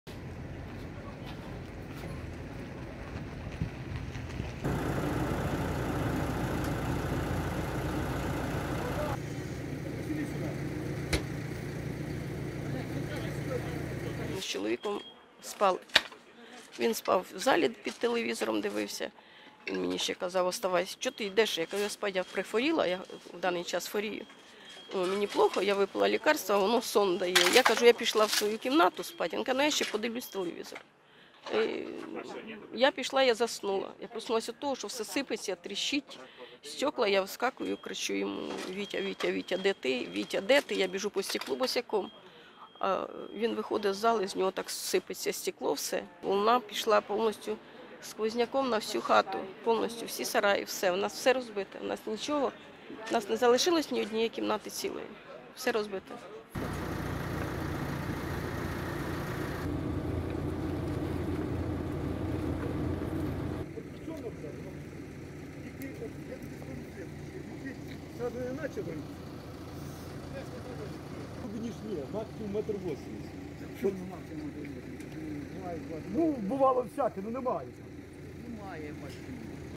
Російські військові обстріляли ракетами передмістя Запоріжжя | Новини
29 грудня близько першої години ночі російські військові обстріляли передмістя Запоріжжя. Жертв та поранених немає. Про це повідомив голова Запорізької ОВА Старух. Ракетами зруйновані кілька будівель, пошкоджені газогін та лінія електропостачання.